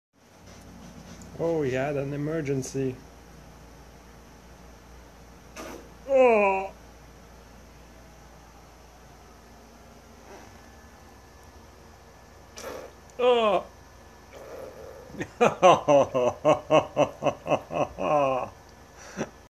cat diarrhea that sounds like a ketshup bottle
The red bucket is because the previous shot did spray outside of his liter box, so I had to clean the carpet.
When that happens, I was cooking hamburger and when I did put some ketshup on it, the bottle did the same farting noise.
The smell was so horrible.